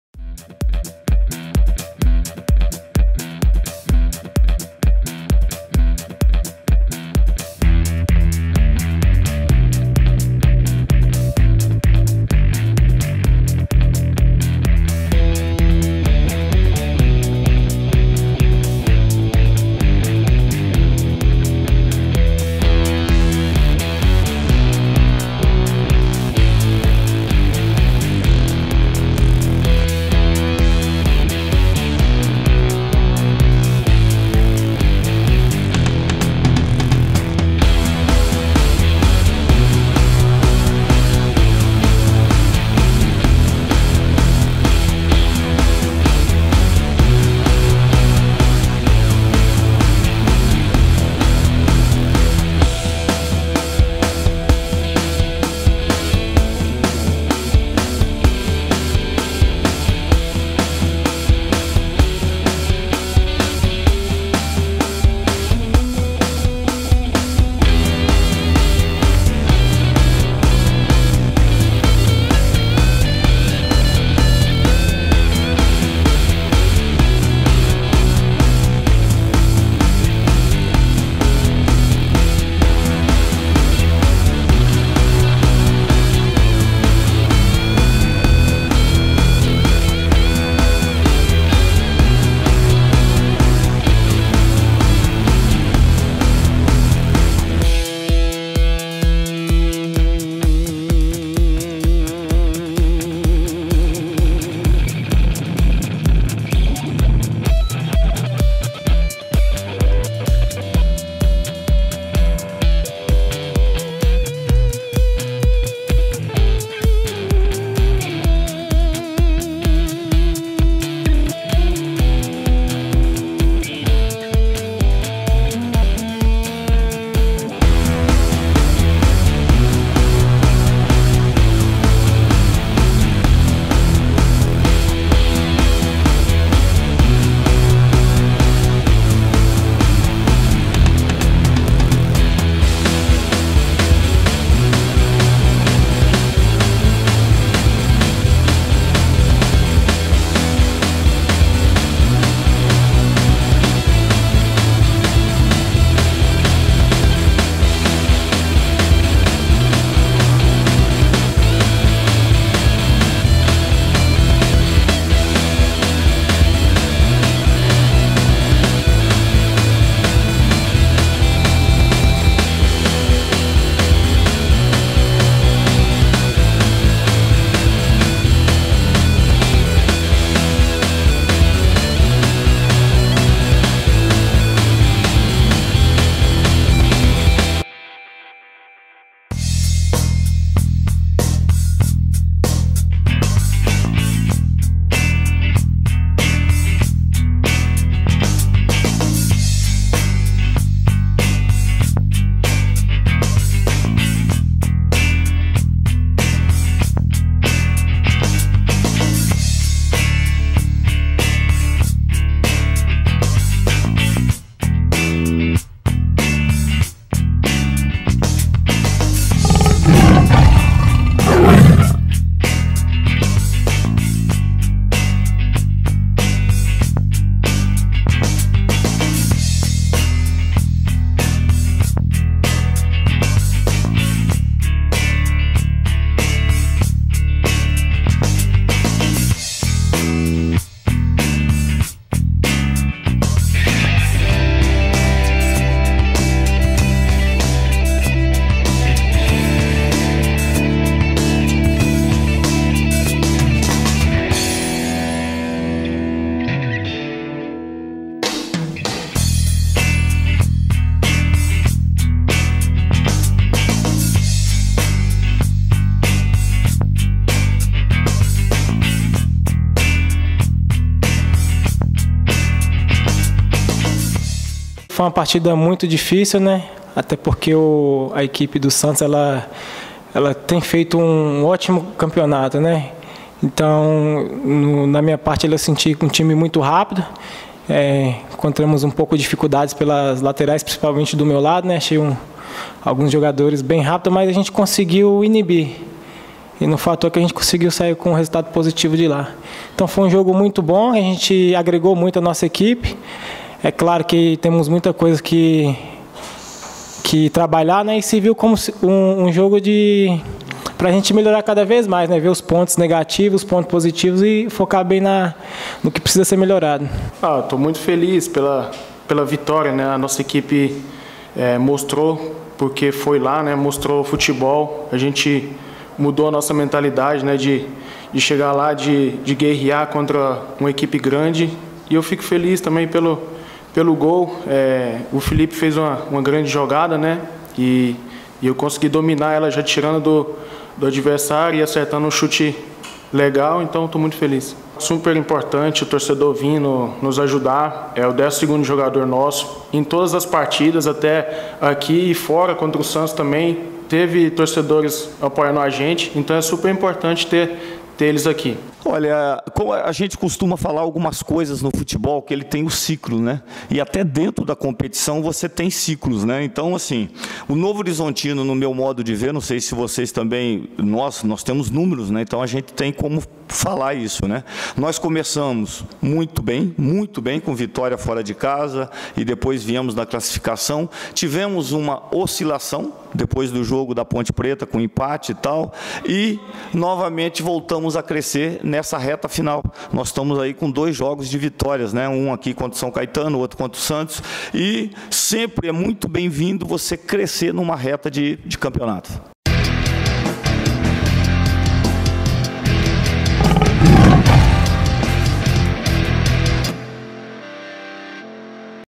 Paulistão 2019 | Rodada 11 - Santos 0 X 1 Novorizontino | Melhores Momentos
A TV Novorizontino é o canal oficial do Grêmio Novorizontino no YouTube. Conteúdo exclusivo para você torcedor do Tigre.

Imagens: Marlon Chiarello, Julio Kenaifes e Thomaz Jannuzzi
Edição: Gustavo Ribeiro